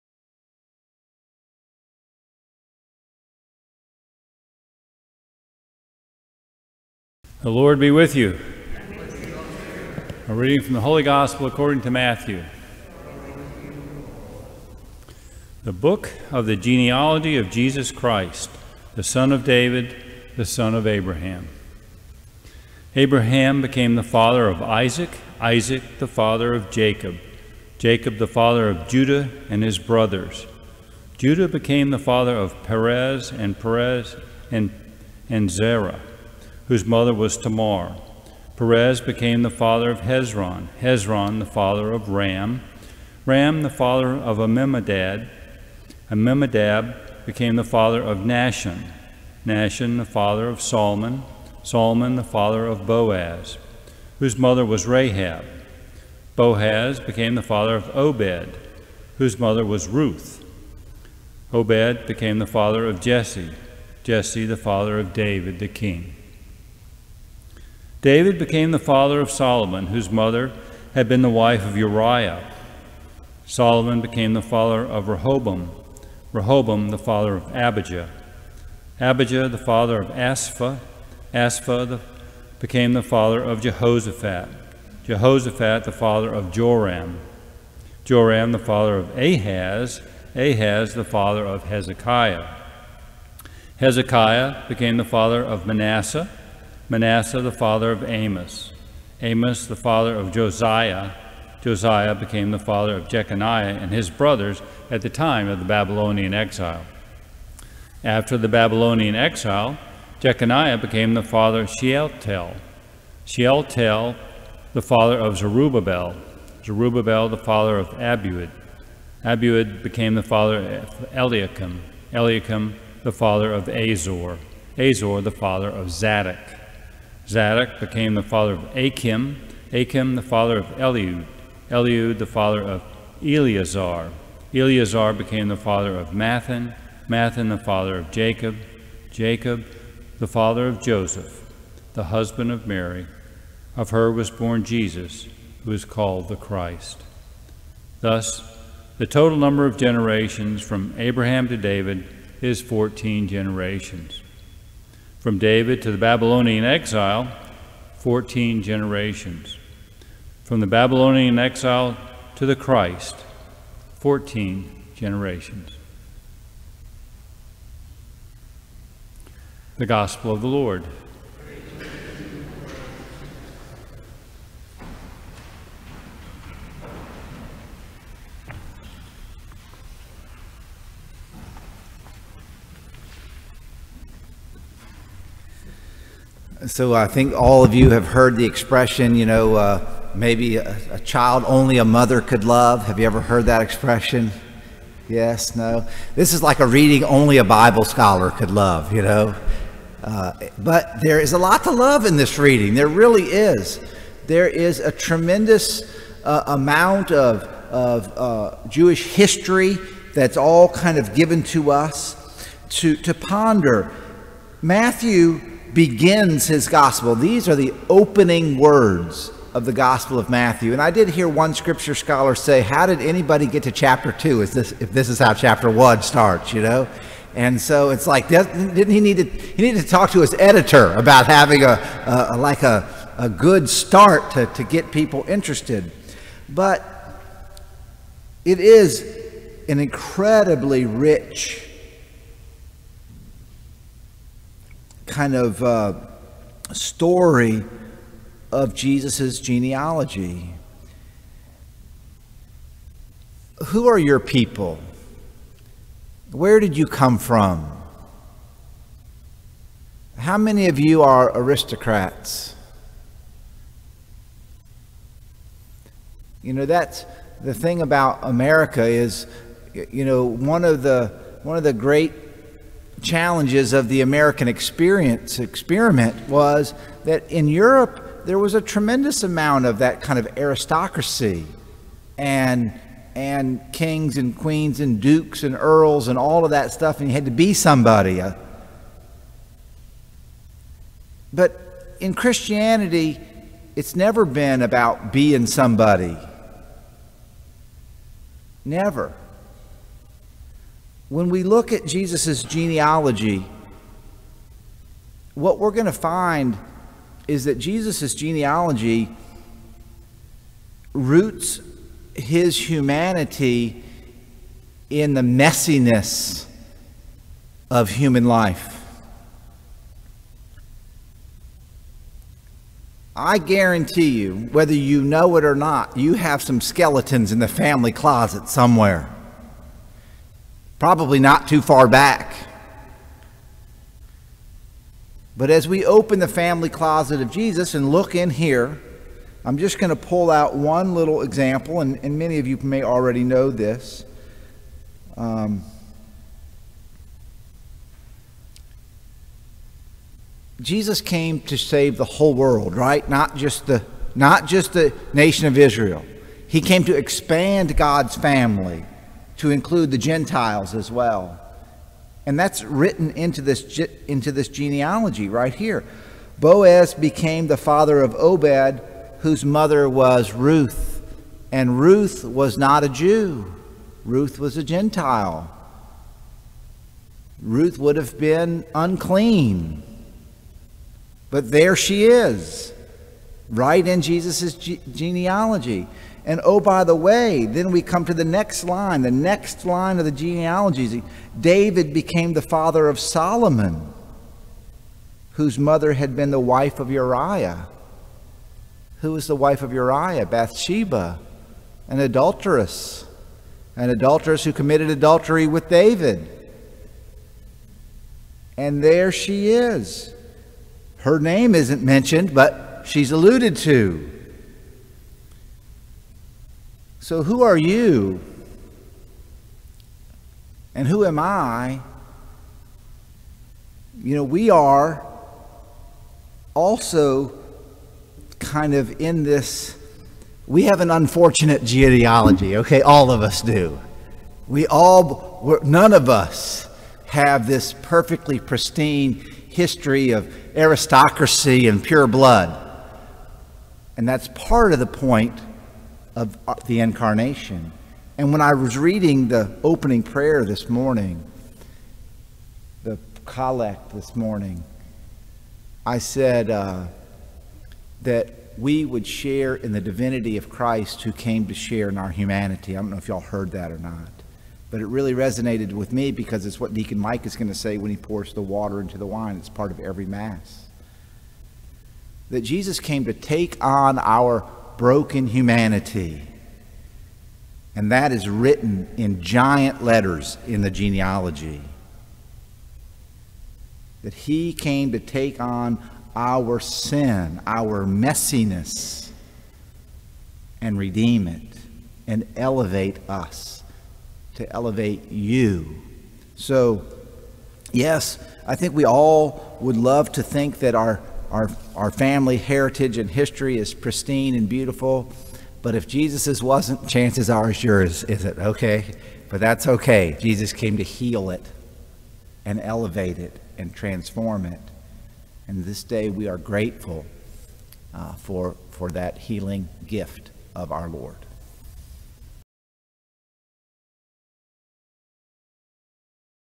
Jesus came to take on our broken humanity.
Father Michael O'Connor's homily from Mass on Wednesday, December 17, 2020 at Our Lady of the Gulf Catholic Church in Bay St. Louis, MS